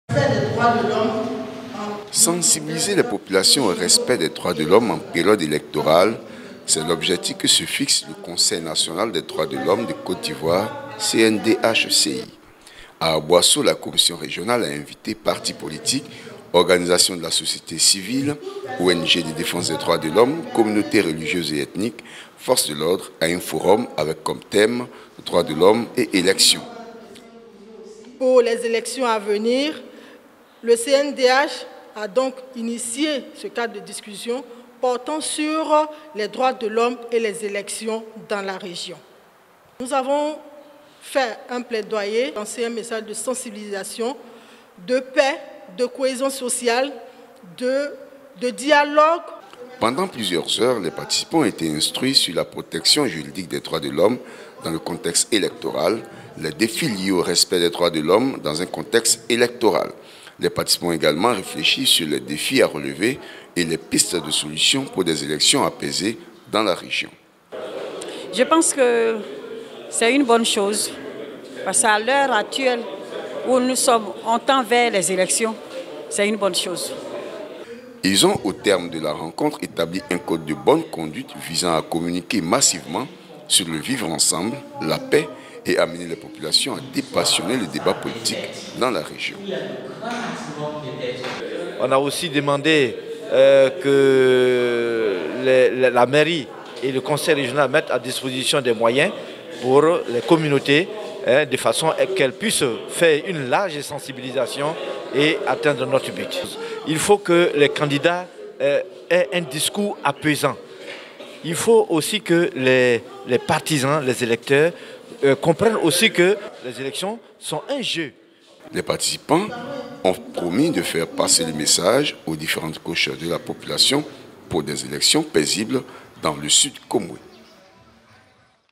Séquence 01
Sensibiliser les populations au respect des droits de l’homme en période électorale, c’est l’objectif que se fixe, le conseil national des droits de l’homme de Côte d’Ivoire (CNDH-CI).
A Aboisso, la commission régionale a invité partis politiques, organisation de la société civile, ONG de défenses des droits de l’homme, communautés religieuses et ethniques, forces de l’ordres à un forum avec comme « thème droits de l’homme et élections ».
Les participants ont promis de faire passer le message aux différentes couches de la population pour des élections paisibles dans le Sud-Comoé